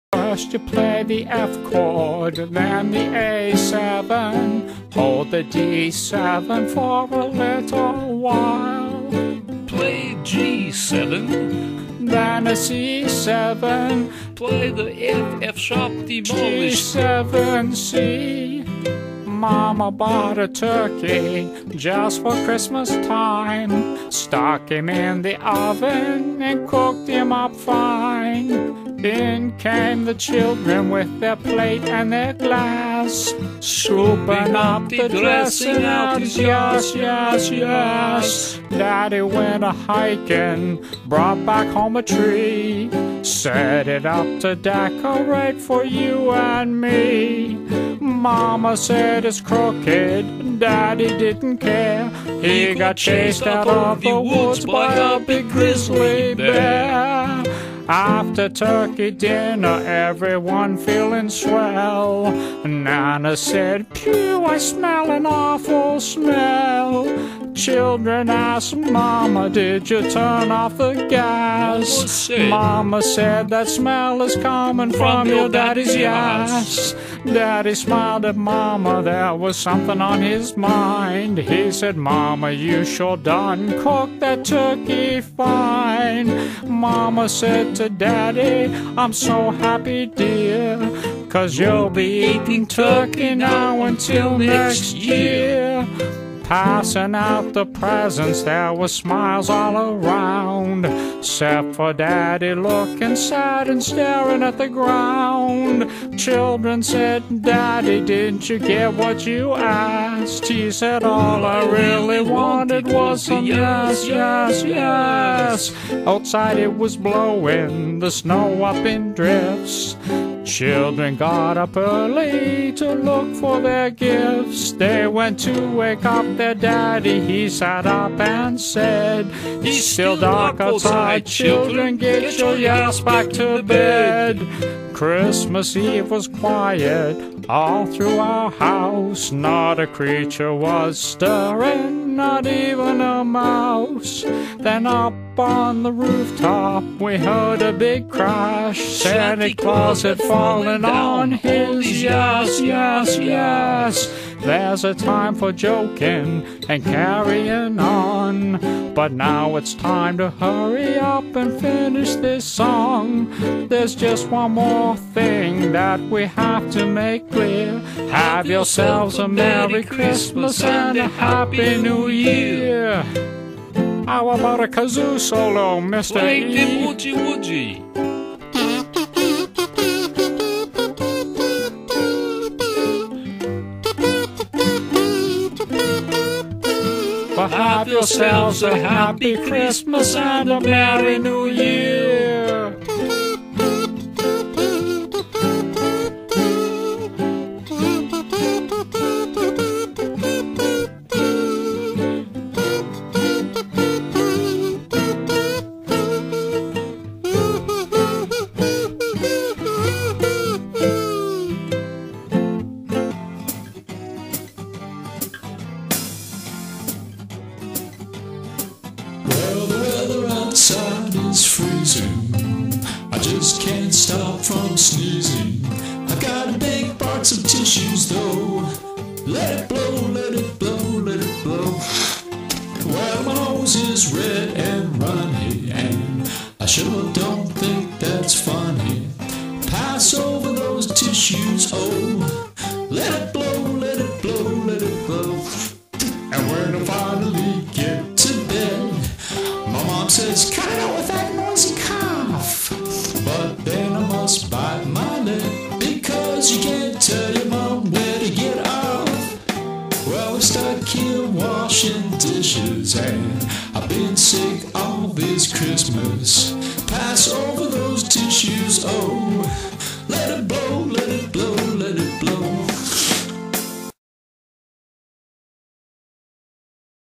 CHRISTMAS UKE TUNE 2010.wmv
Part of a video ukulele lesson I created for my nieces in Calgary for Christmas 2010. Featuring yours truly, "Uncle Mark, and the first appearance of the mysterious "Mr. E".